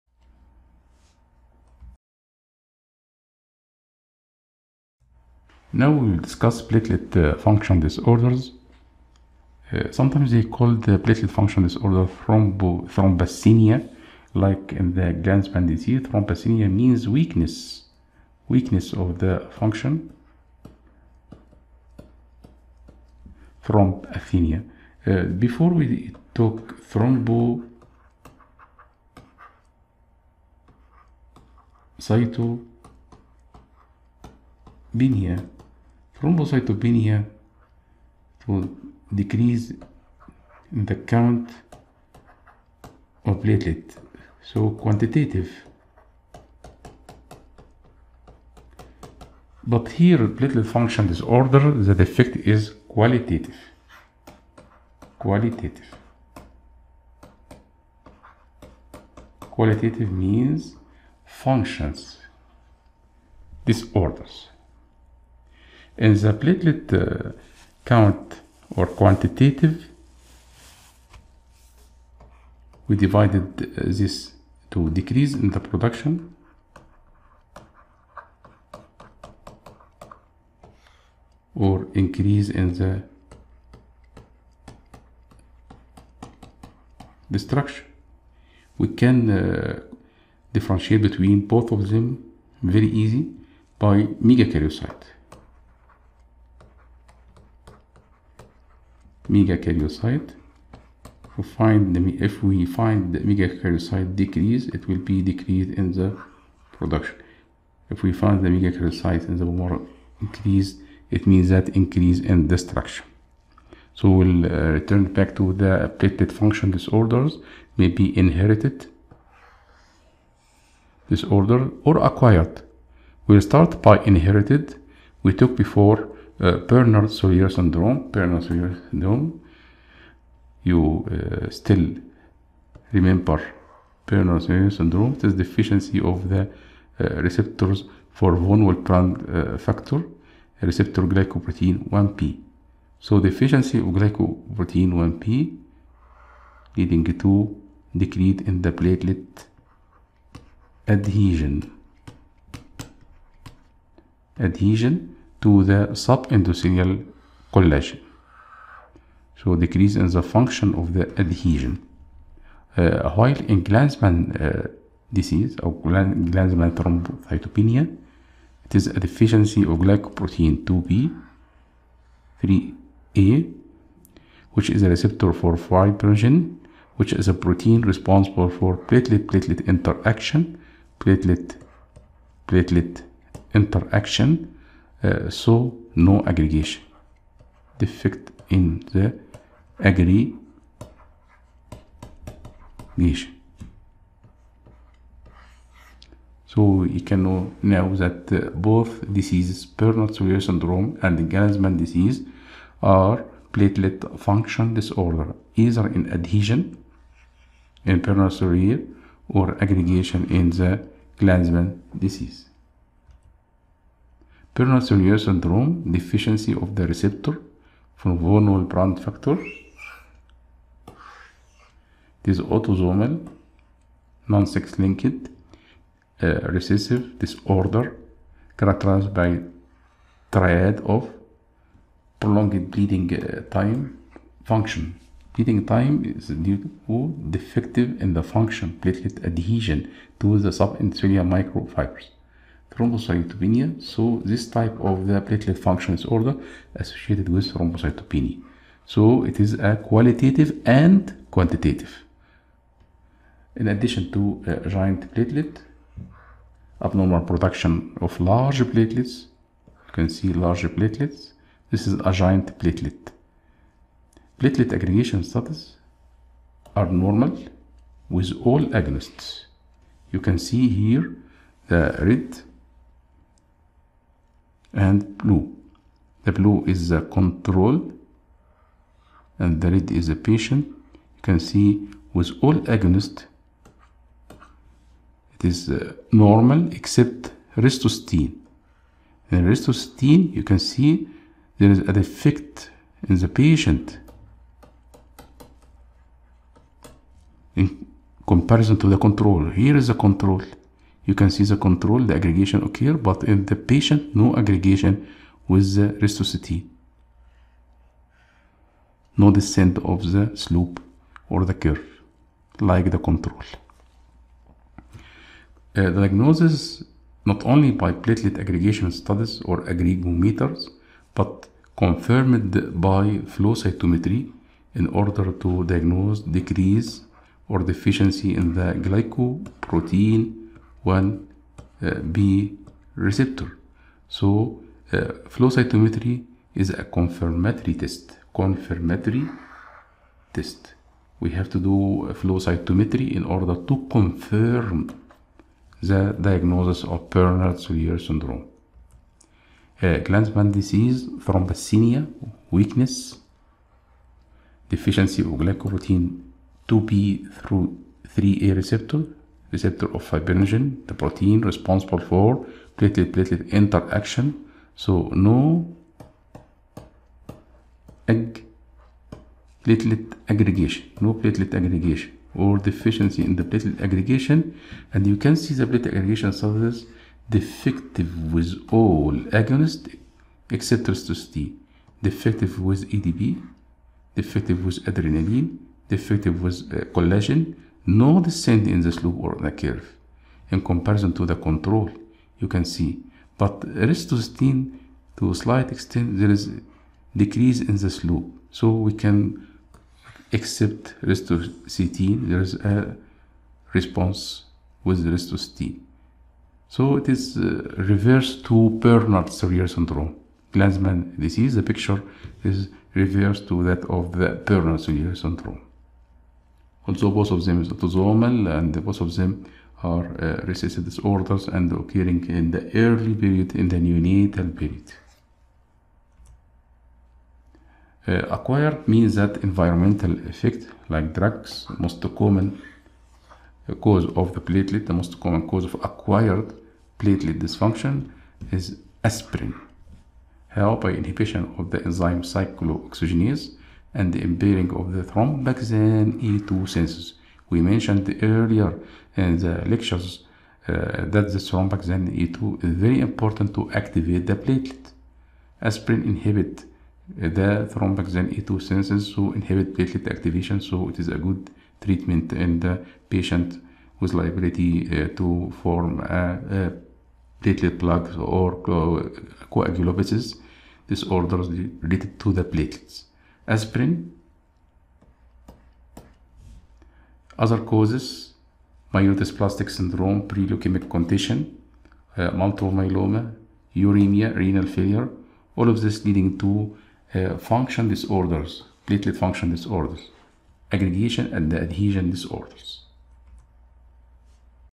Pathology Hematology | 45 | Thrombathenia
الوحدة 2 : Coagulation Disorders
الدرس 45 : Thrombathenia
1- Glanzman’s disease
2- Bernard Soulier syndrome
3- Acquired platelets functions disorders